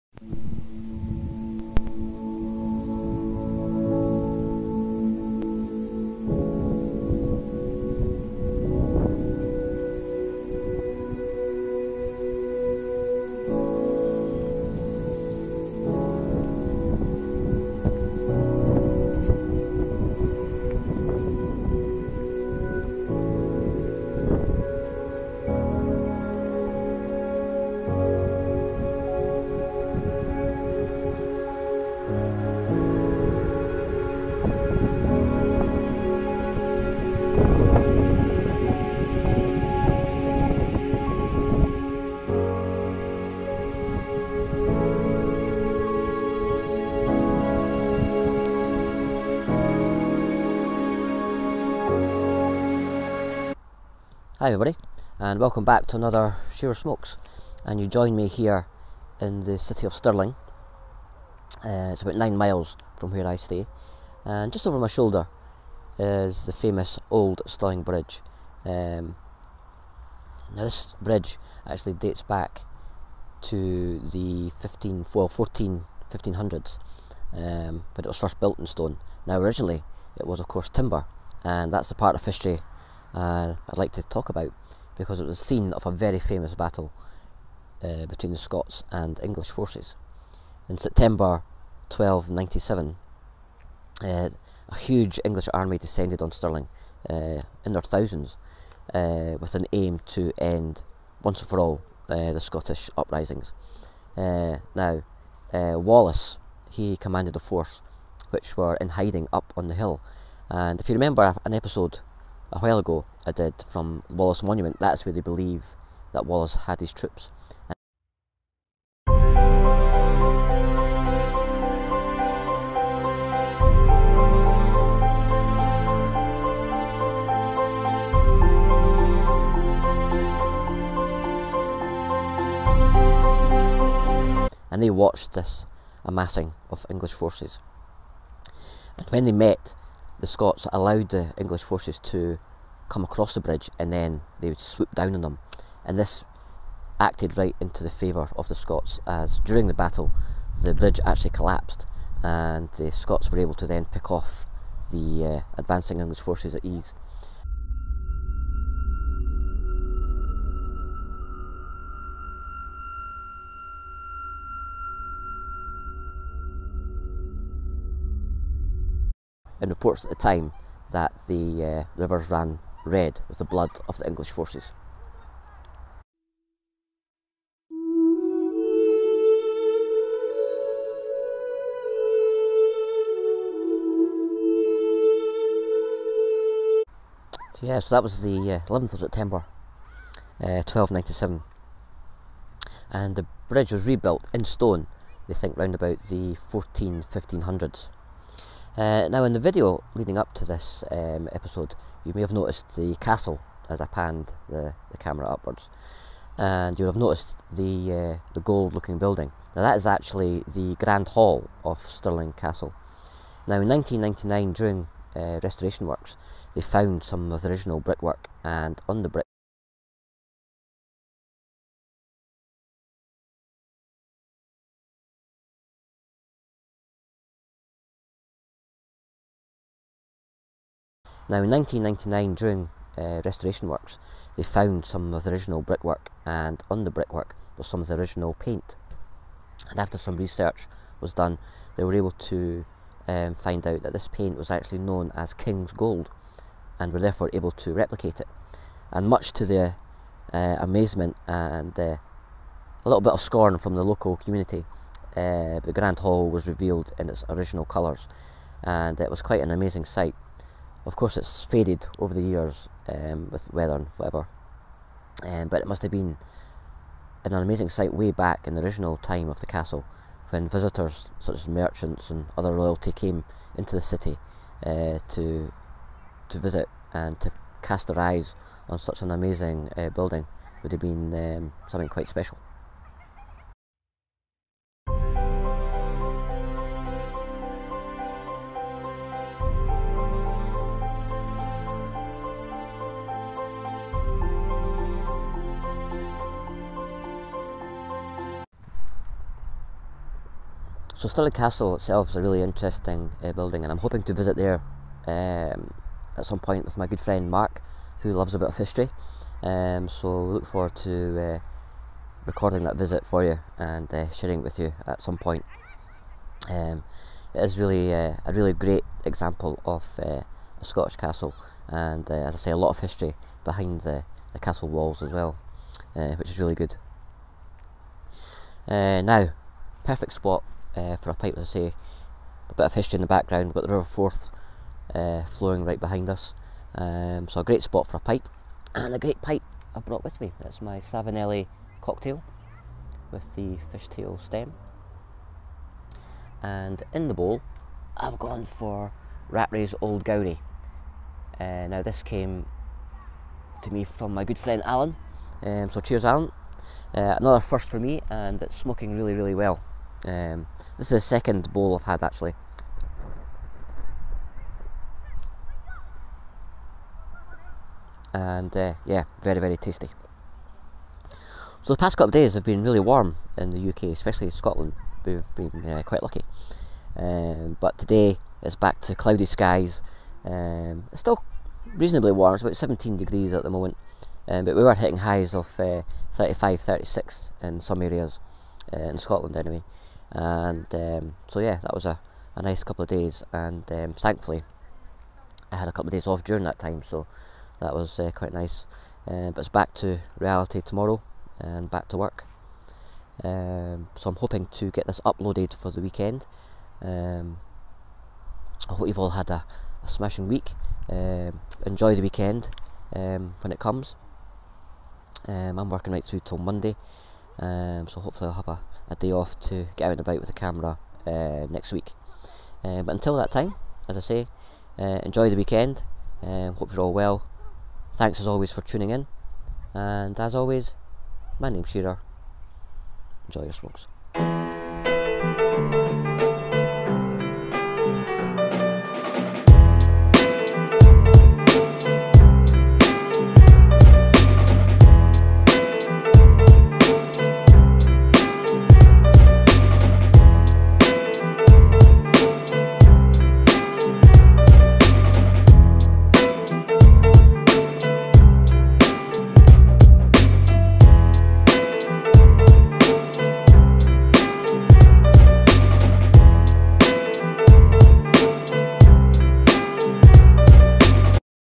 Shearer Smokes @ Stirling Bridge
Old Stirling Bridge where William Wallace fought the English forces in 1297
Savinelli Cocktail with Rattrays Old Gowrie